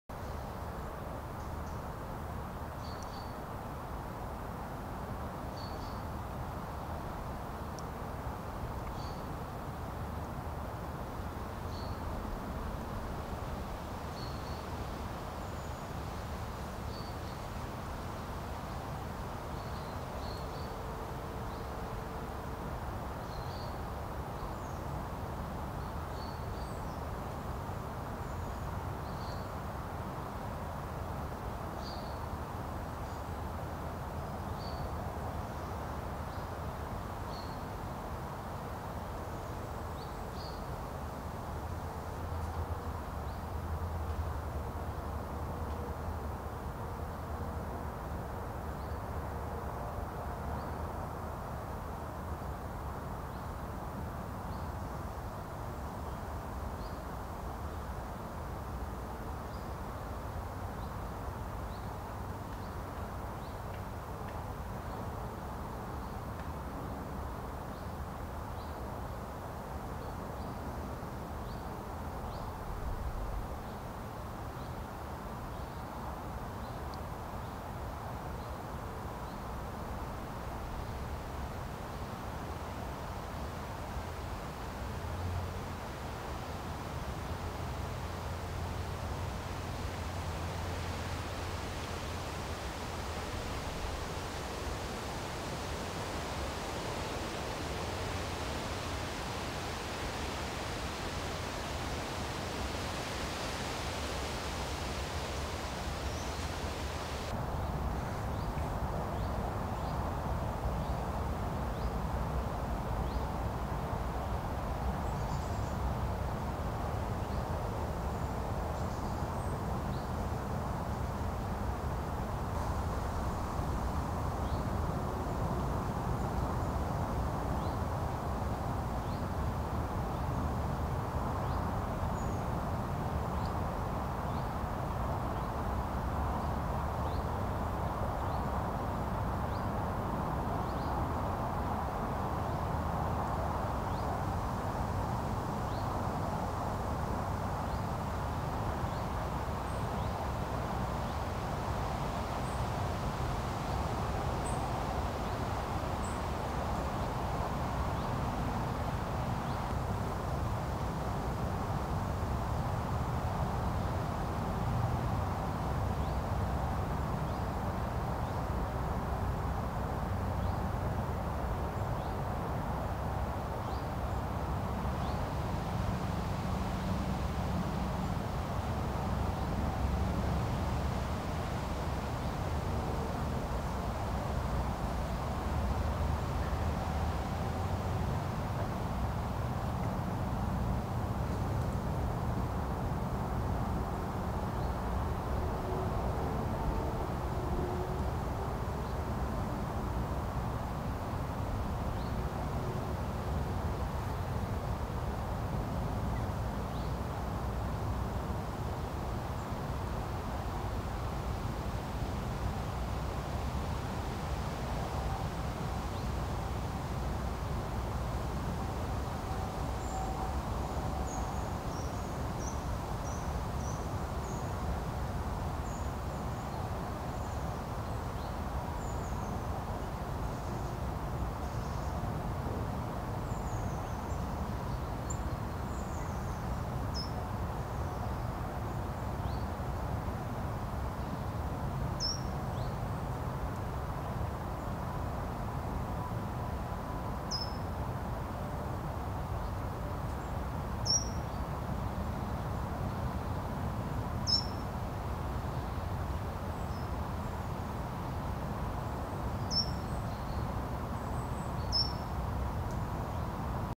A larva of a WaSP eat the style sheet 🔴
Description (more): A larva of a sheet WaSP leans on the fruit of the lysimachia Vulgaris and eat the remains of the style. The larva gnaws through the middle of the style and eat, which to a large extent on.